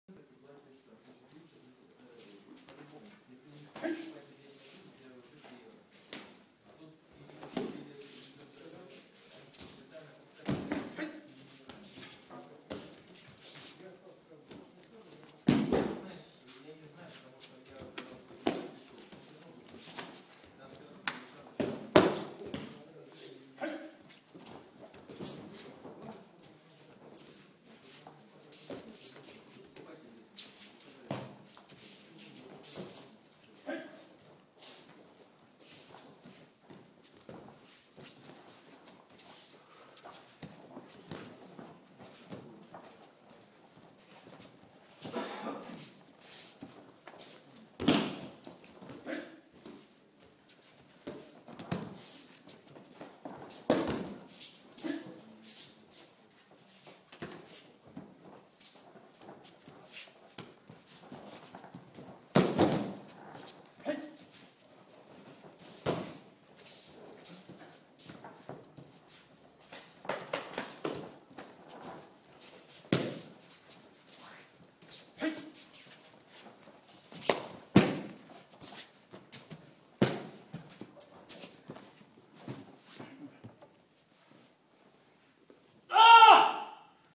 рандори в гайдзин додзё
Рандори - традиционное завершение тренировки Айкибудо. Полученные на тренировке знания по выбранным техникам применяются в рандори. Где на выполняющего технику нападают два человека. Удары и хваты могут быть произвольными или заранее оговоренными. Задача выполняющего применять технику и следить за ее чистотой. В айкибудо техника достаточно травматична, поэтому искусство состоит не только в правильном исполнении техники айкибудо, но аккуратности, заботе по отношению к партнерам.
Материал принадлежит Межрегиональной Федерации Айкибудо